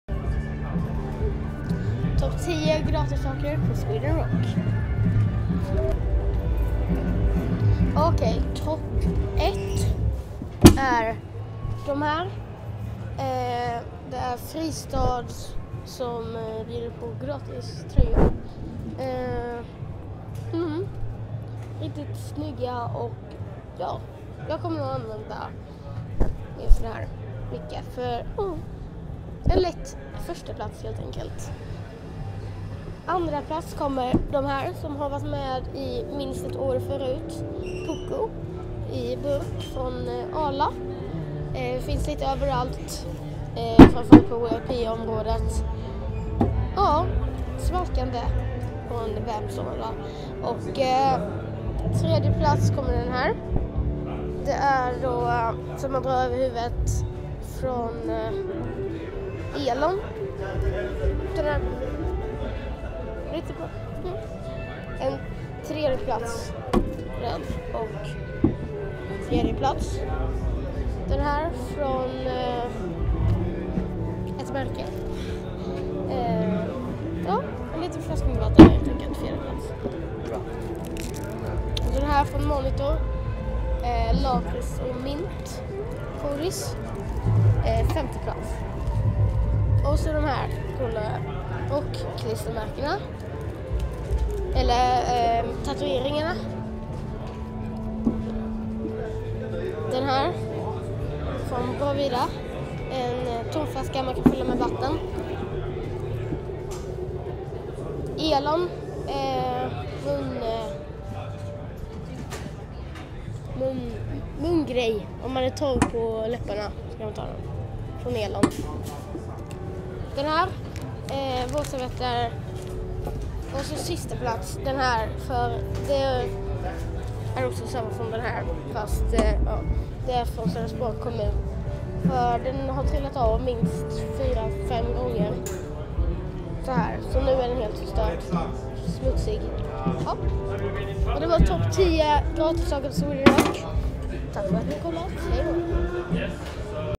10 BÄSTA GRATISFYNDEN PÅ SWEDEN ROCK 2019 (BLT)
En film jag gjorde på Sweden Rock, fredag den 6 juni 2019.
Filmen är för BLEKINGE LÄNS TIDNING - BLT.